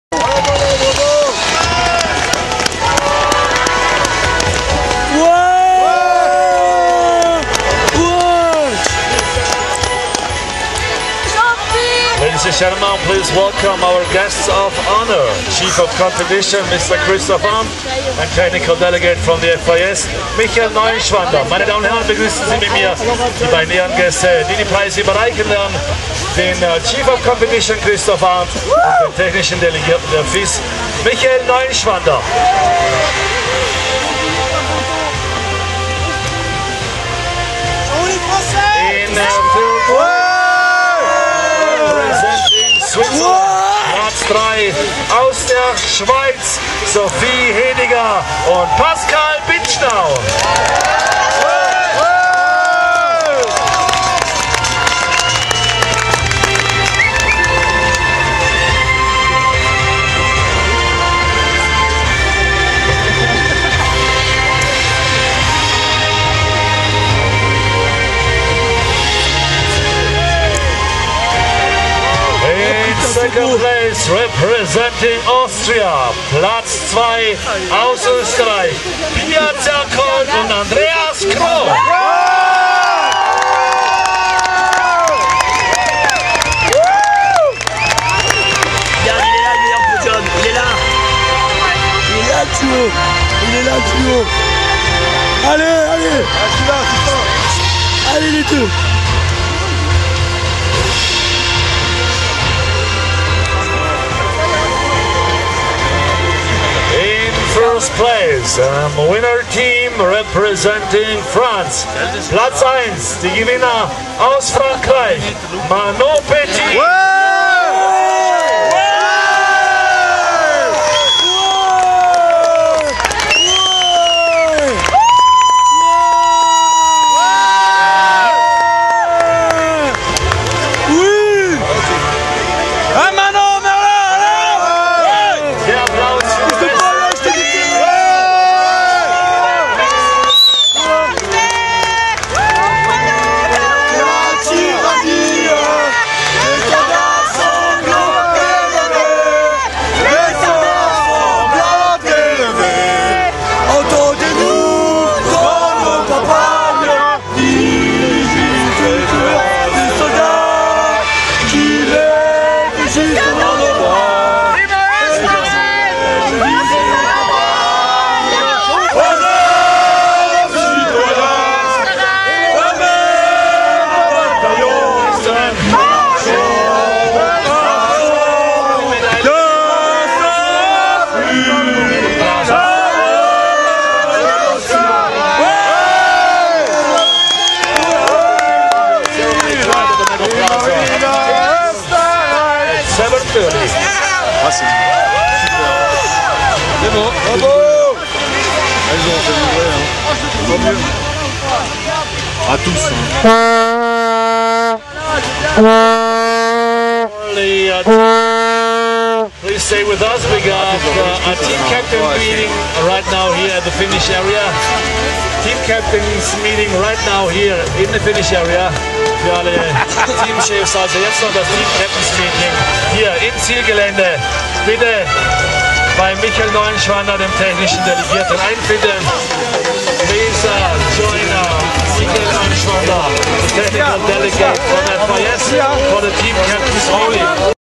Podium Team Event Piste EYOF 2015
Victoire de l'équipe française de snowboardcross EYOF 2015
Manon Petit et Merlin Surget